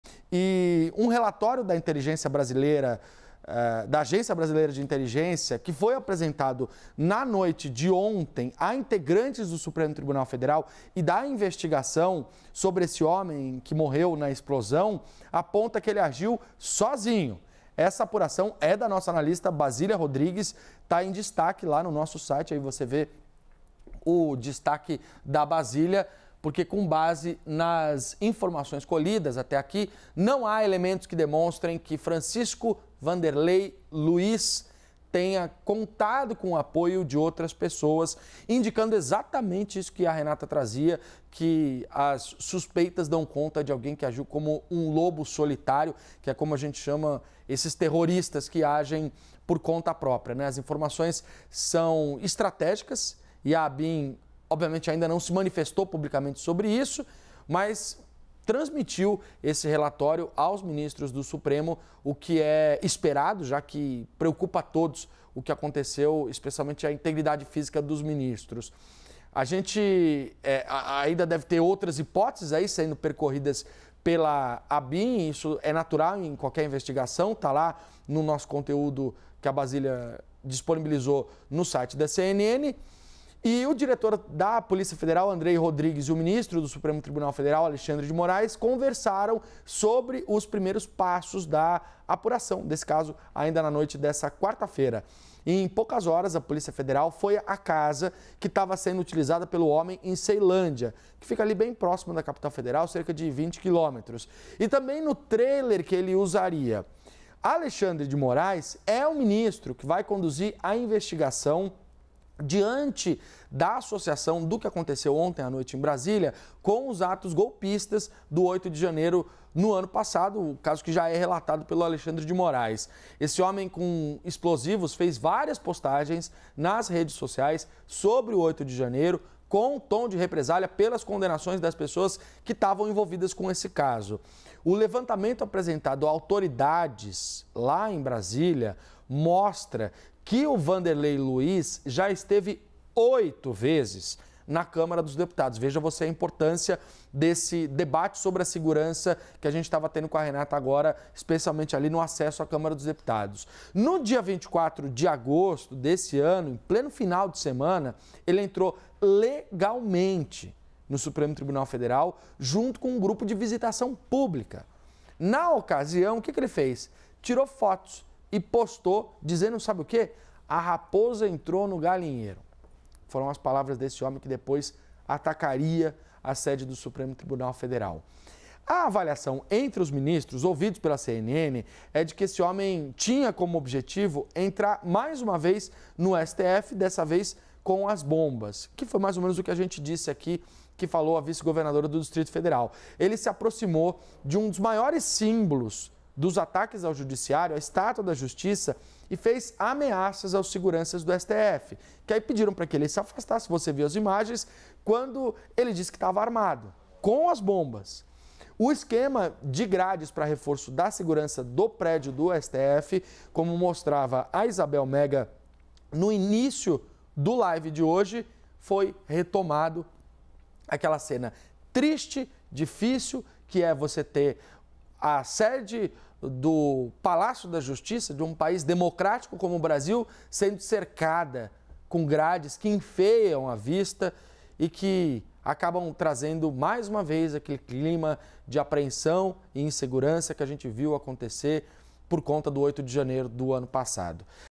Relatório da Abin aponta que homem agiu sozinho, afirmam integrantes do STF | LIVE CNN
Um relatório da Agência Brasileira de Inteligência (Abin), apresentado na noite desta quarta-feira (13) a integrantes do Supremo Tribunal Federal e da investigação sobre o homem que morreu em explosão, mostra que ele agiu sozinho. Com base nas informações colhidas até aqui, não há elementos que demonstrem que Francisco Wanderley Luiz tenha contado com apoio de outras pessoas. A informação é de Basília Rodrigues.